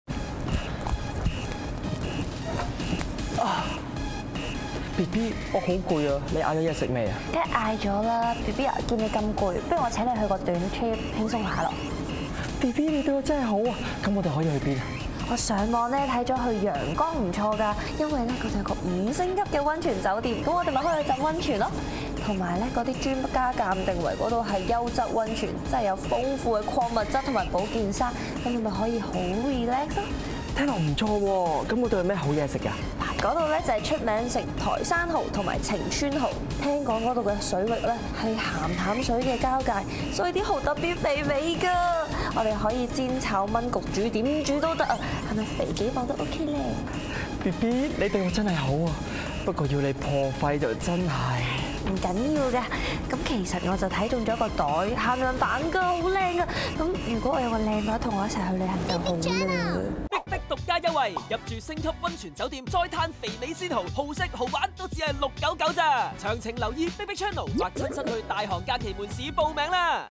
【蠔氣溫泉勁爽團 超值$699】 溫泉蠔爽食玩團 | 港珠澳大灣區陽江旅行團 | 大航假期 | 網購攻略第54集
大航假期 港珠澳大橋 勁爽大灣區旅行團 | 陽江旅遊 | 皇家酒店溫泉旅行團 | 食蠔美食團 | $ 699起 ：

$ 699 就可蠔門酒店溫泉純玩2天，豪歎蠔食5餐 超值吖喂 🤤！大航假期特別推出大灣區旅行團，港珠澳大橋豪華巴士香港接送更便捷～滋味品嚐肥美鮮嫩嘅蠔中珍品，蠔氣沖天盛宴包你食過難忘；任浸頂級皇家酒店38個空中溫泉，美容健康養生必遊；特色打卡美景如畫嘅禾海稻浪，盡享遠離凡囂嘅喜悅！啱哂想要休閒美食嘅你，和全家大細、親朋戚友一齊放鬆身心喇❤！